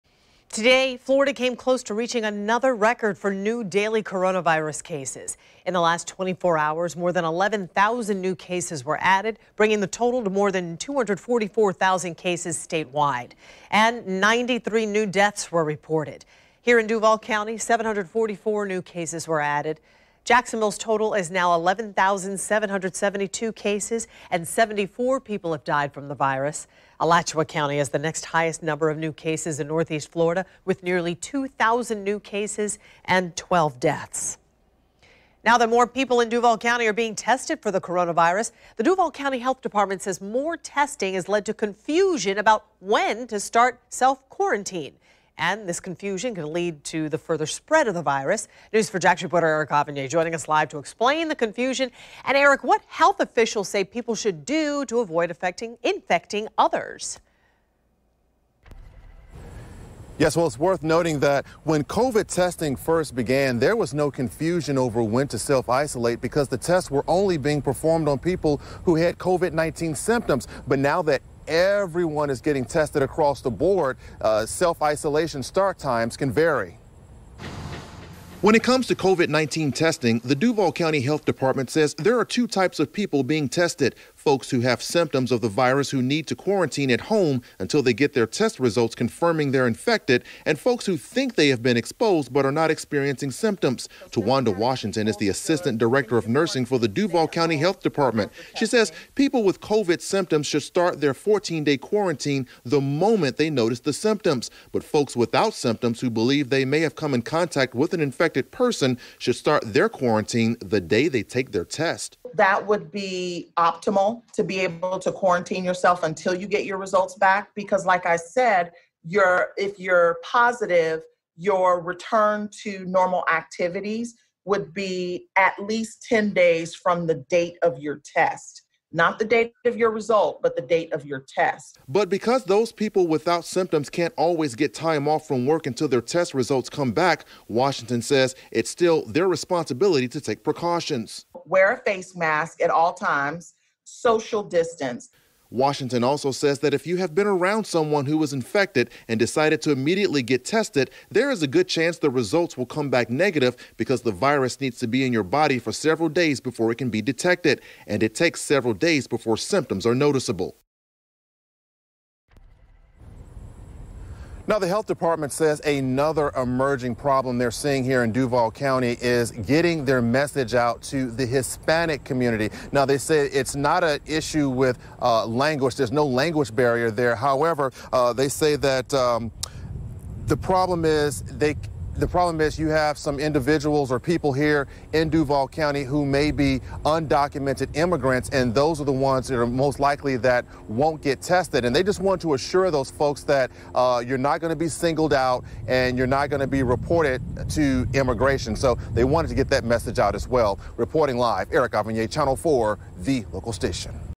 DOH determing when to self-isolate causing confusion
According to the Duval County Health Department, more testing has led to confusion regarding when to self-quarantine.